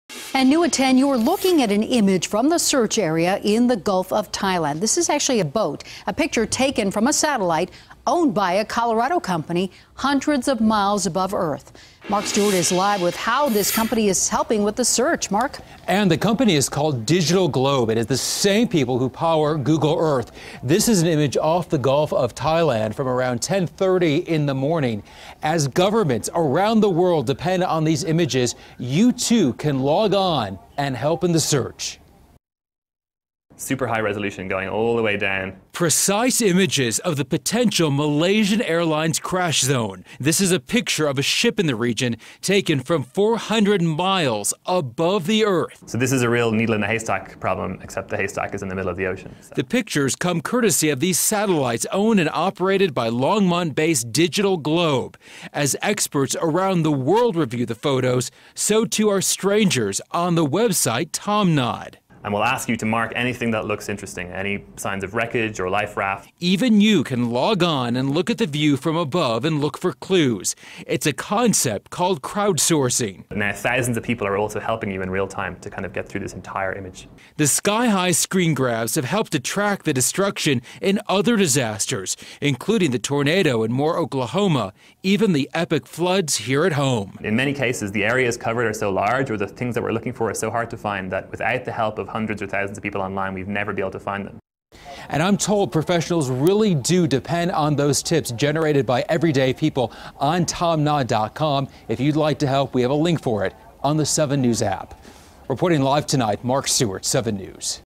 Longmont company helps in search for missing jet
A Longmont company is providing satellite images to help in the search for the missing Malaysian Airlines plane.